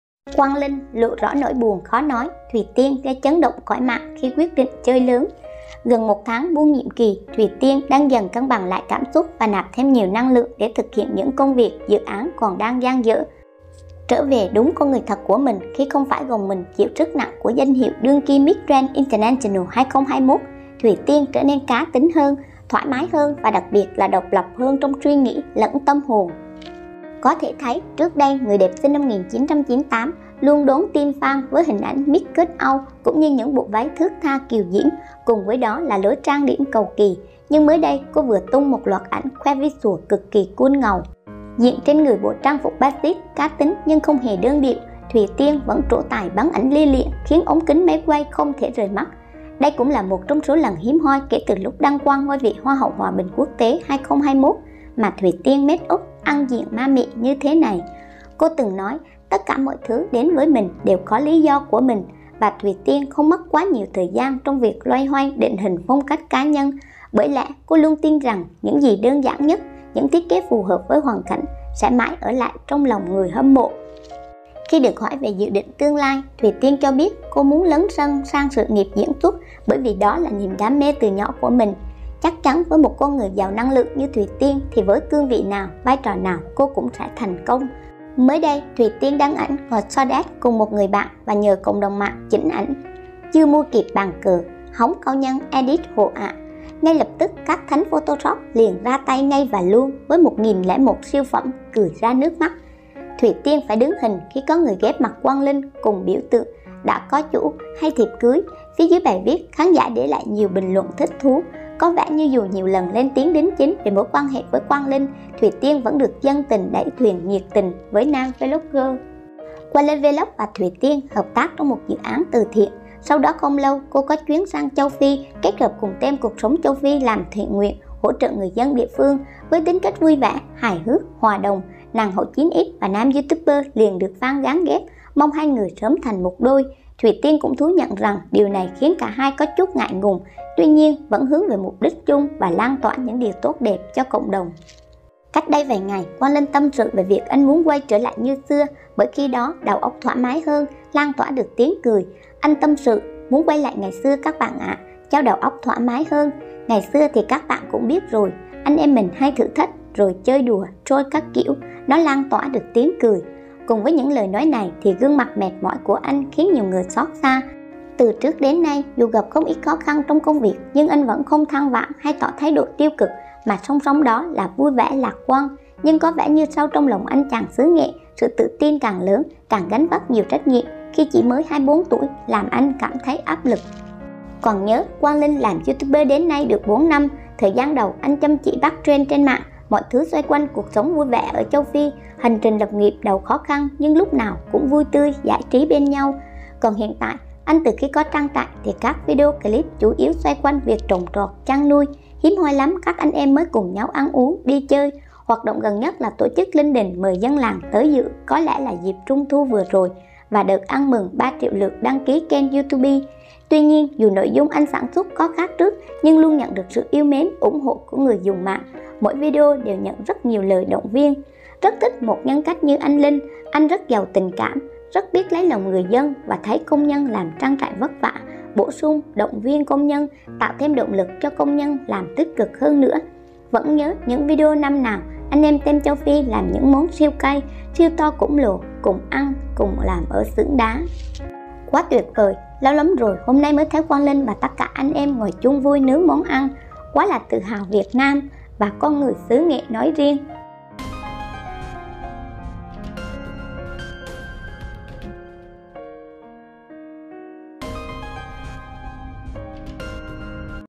Quang Linh lộ rõ nỗi buồn khó nói, Thùy Tiên gây chấn động cõi mạng khi quyết định ‘chơi lớn’
Gần 1 tháng buông nhiệm kỳ, Thùy Tiên đang dần cân bằng lại cảm xúc và nạp thêm nhiều năng lượng để thực hiện những công việc, dự án còn đang dang dở.